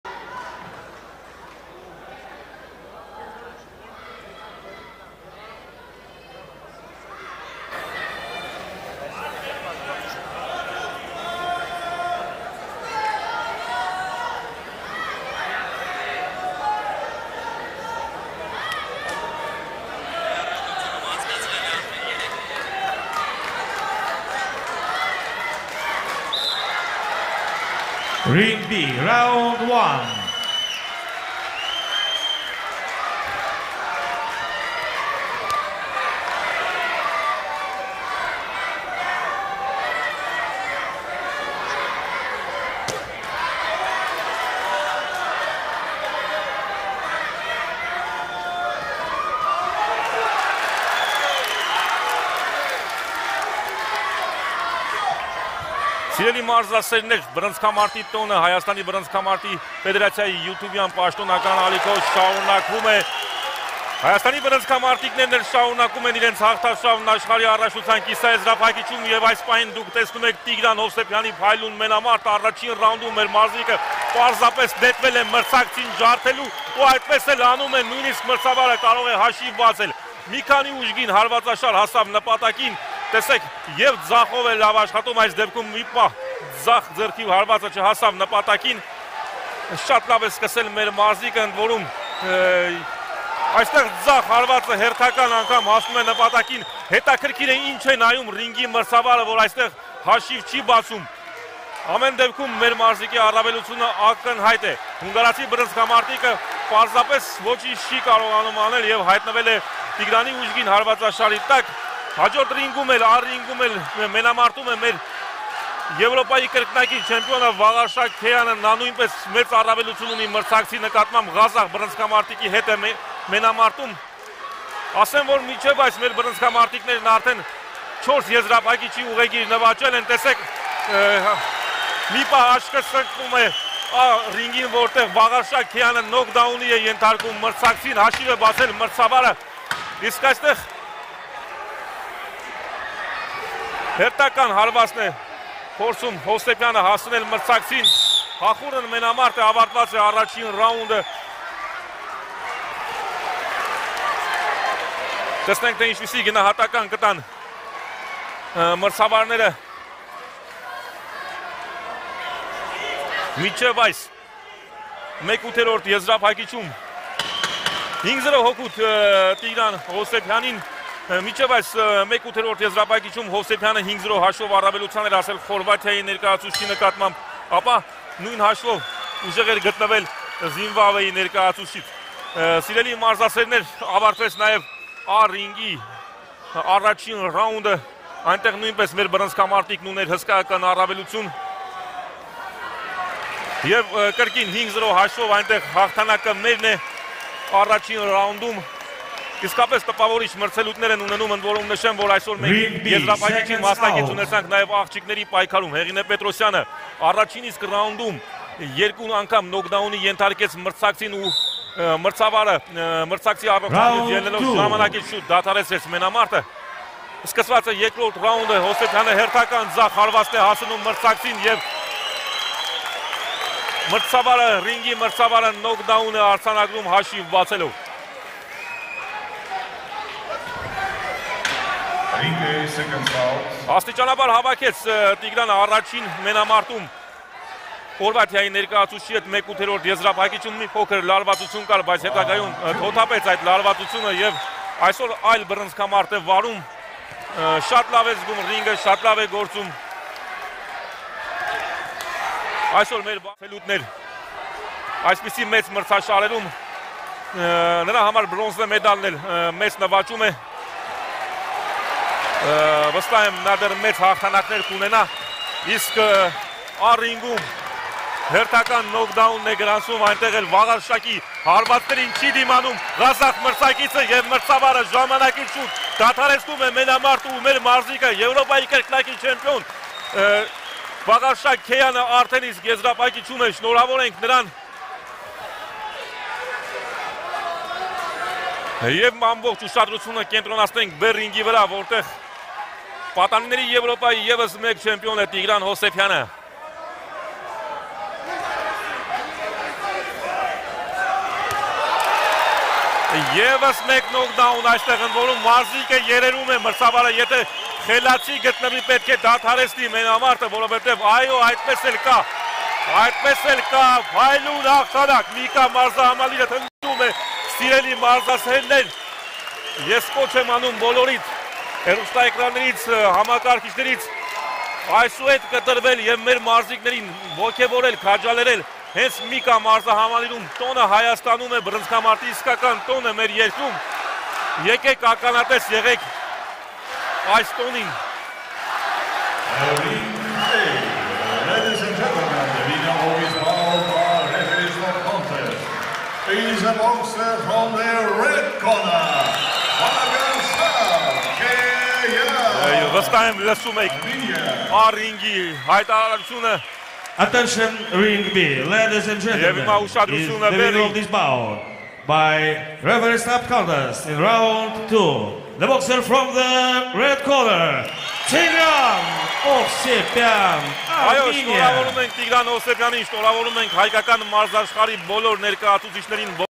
🇦🇲🆚️🇭🇺/Տիգրան Հովսեփյանը 2 ռաունդում լուծում է հունգարացու հարցերը ու դուրս գալիս ԱԱ-ի եզրափակիչ
🥊  Բռնցքամարտի պատանիների աշխարհի առաջնություն